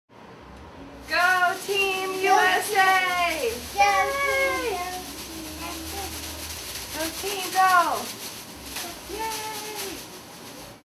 Amy is cheering for U. S. A.
Amy is cheering from Portland, OR